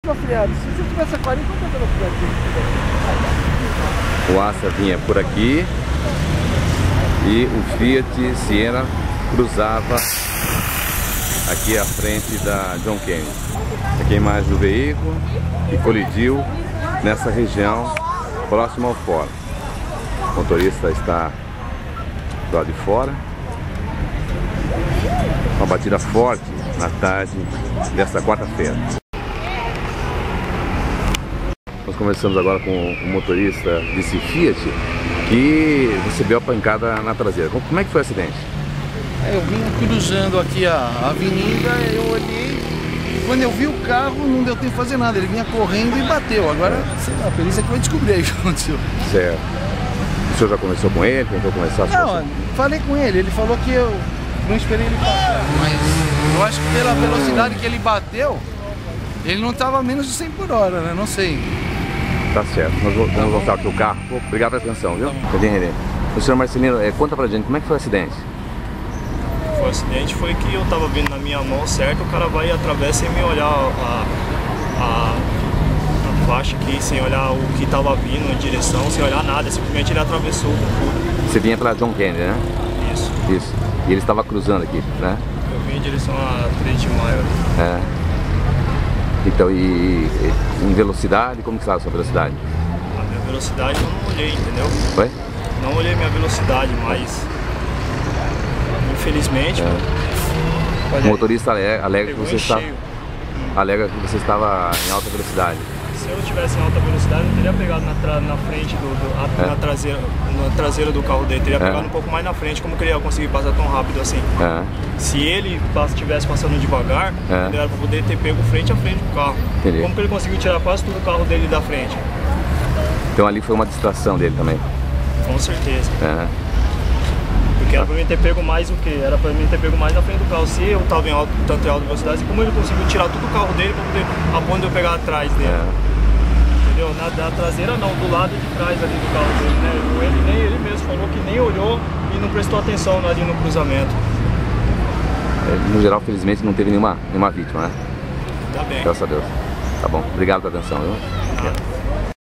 Dois veículos colidem na Avenida John Kennedy em São Roque
No início da noite de quarta-feira, 21/12, dois veículos colidiram na Avenida John Kennedy em São Roque, nas proximidades do Fórum. Felizmente ninguém ficou ferido apesar da forte pancada. Assista a reportagem do Jornal da Economia que realizou a matéria logo após o acidente.